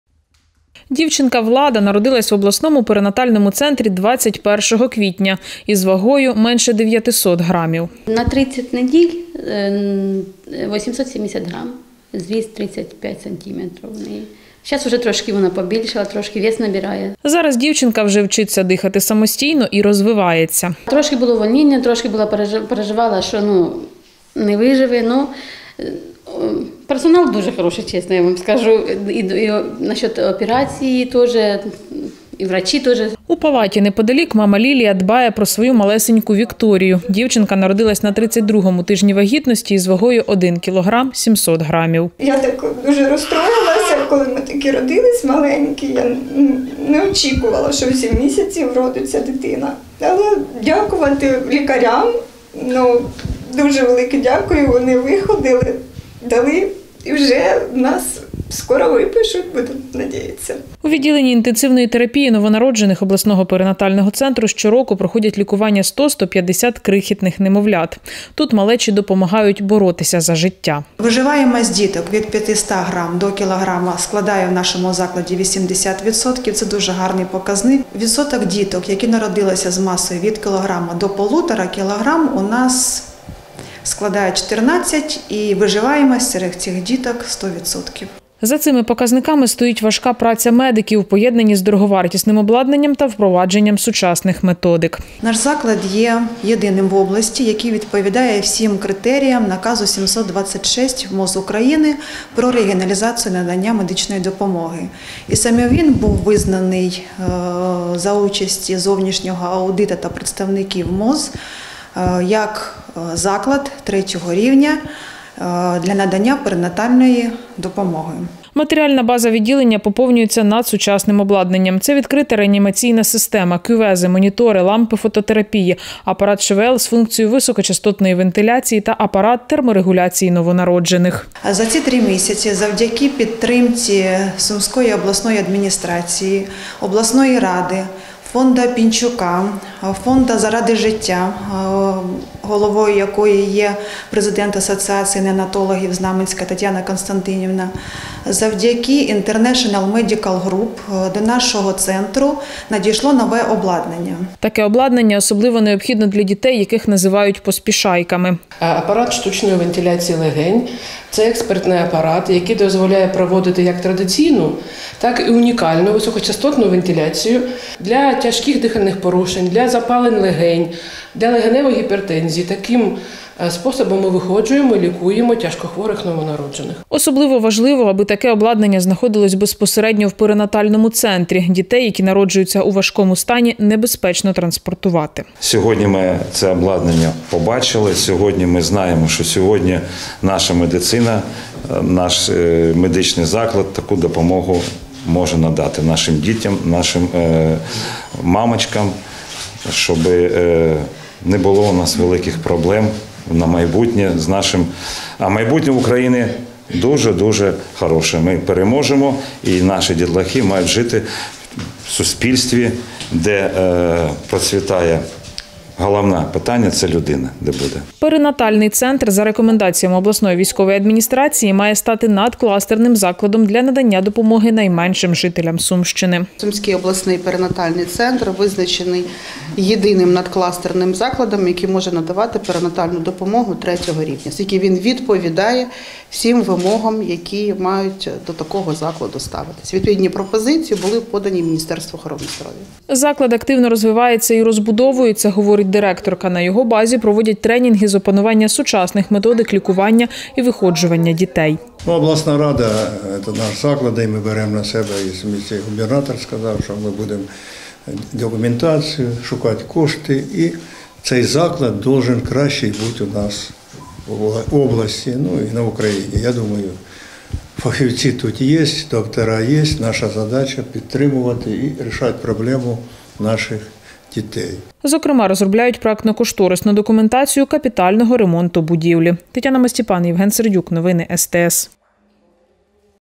Все для порятунку найменших: обласний перинатальний центр отримав сучасне обладнання
Сумський обласний перинатальний центр отримав сучасне обладнання вартістю понад 8 мільйонів гривень. Апарати для реанімації та підтримки здоров’я немовлят, що народились завчасно або з певними порушеннями, надали закладу фонди та організації з різних країн світу.